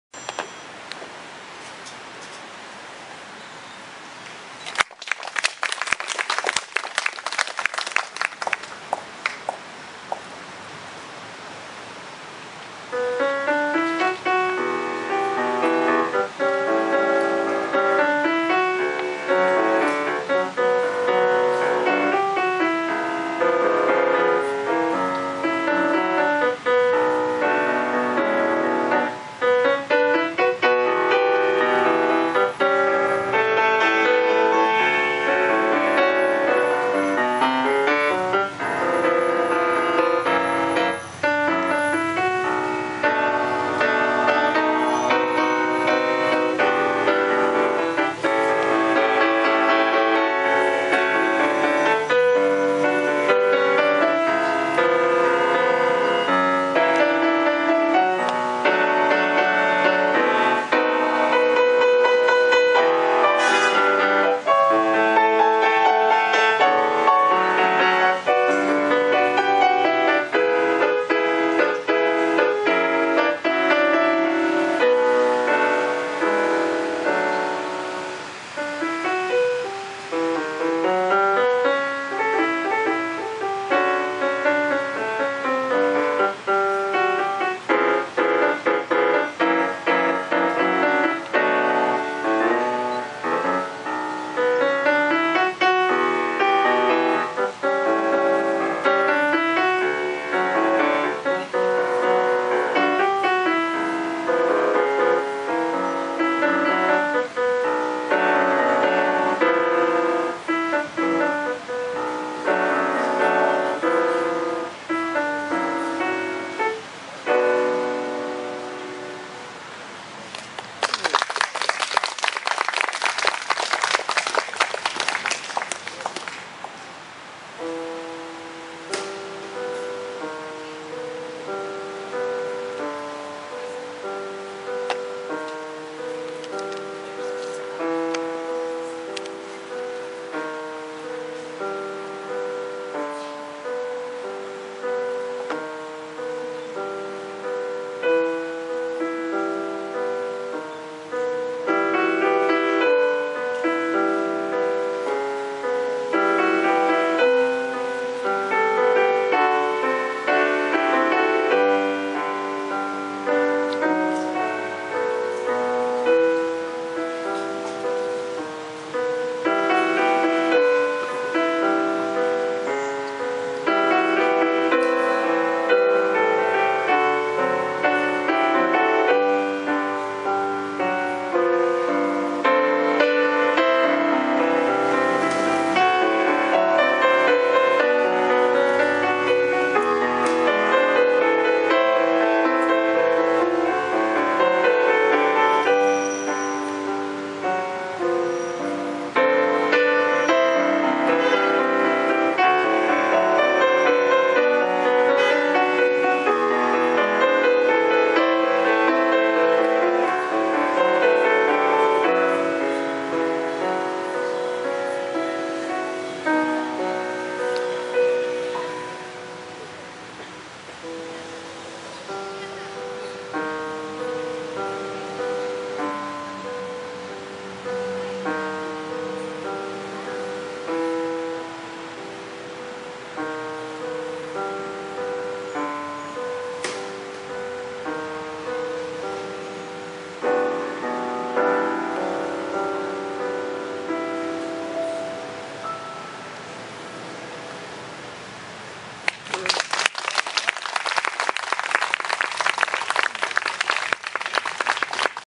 Young pianist Victoria Zemskov at Russian Cultural Garden
Young pianist Victoria Zemskov performed in the Russian Cultural Garden in Cleveland Ohio.  Her teacher is Svetlana Ivanova. She performed on a grand piano at the inaugural Grand Piano in the Russian Garden concert.  The concert's theme was Music of the Cultural Gardens of Cleveland.